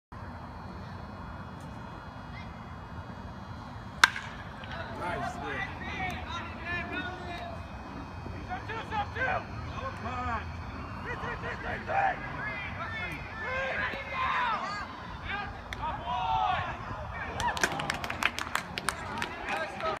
Weston Ellis Triple in 11.80 seconds 2/16/15
Triple in 11.80 seconds to left center wall. Phenom Signature Elite. San Manuel Stadium, San Bernardino, CA February 16, 2015